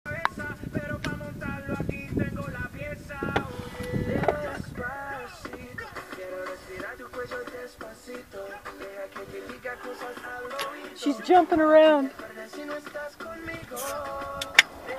WildHorse Baby Jumping
The Wild River, San Luis wild horses that hang out by the highway heading into Colorado from New Mexico had a little one this spring and it must be within a day old.  The first time it jumped it fell down.  It was so dang cute so I kept my camera on her and sure enough she jumped again, but stayed upright swishing her tail like crazy...